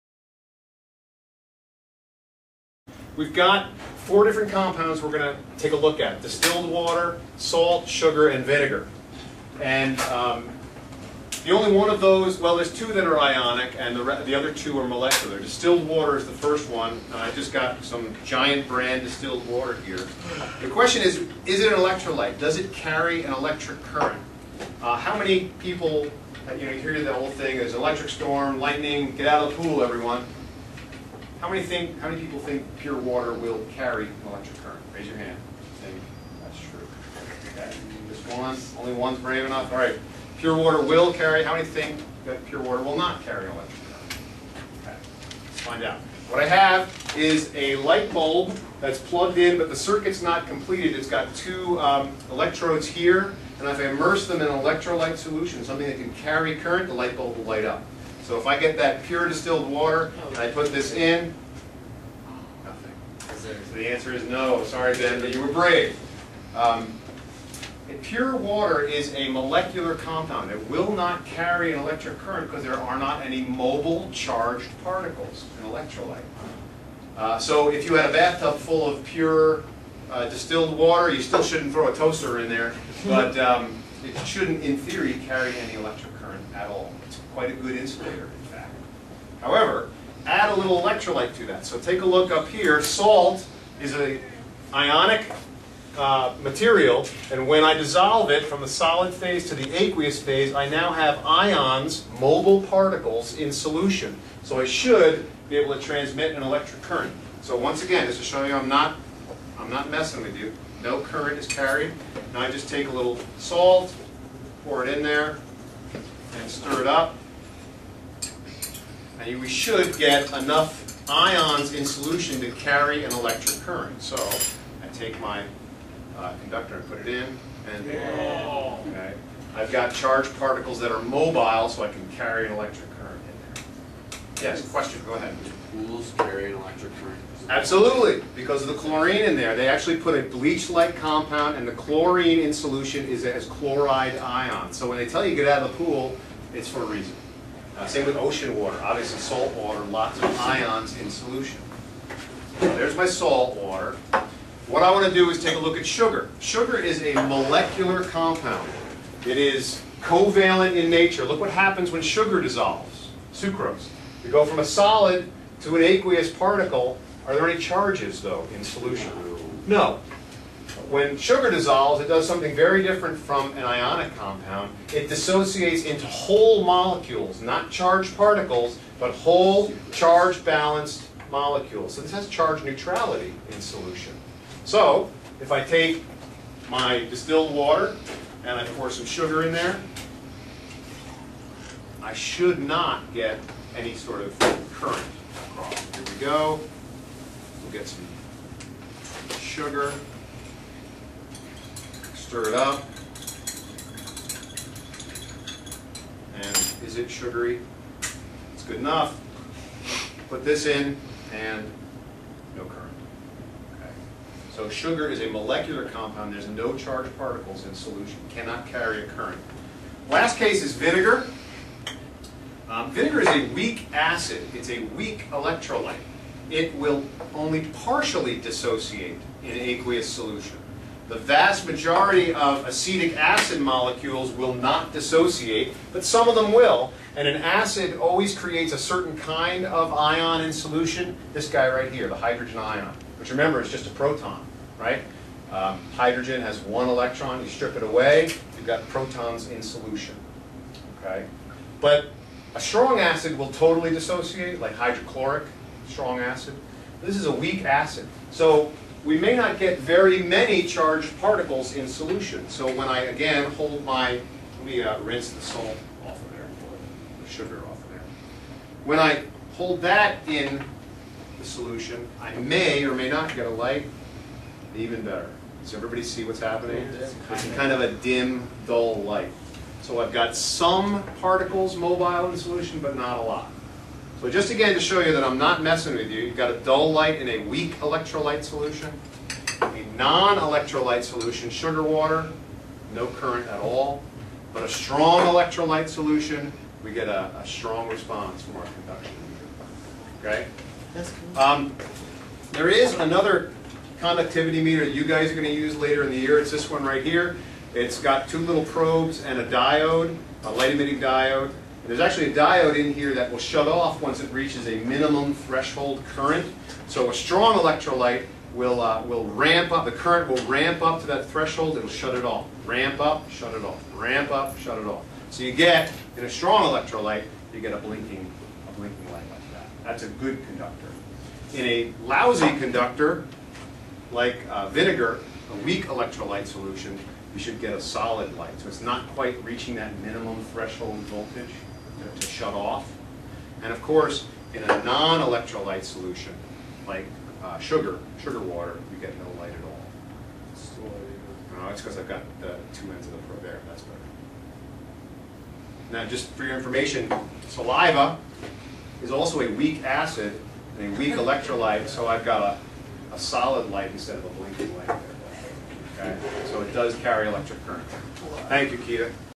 Strong, Weak, and Non-Electrolytes
A demonstration illustrating the conduction of an electric current by charged particles (ions) in solution.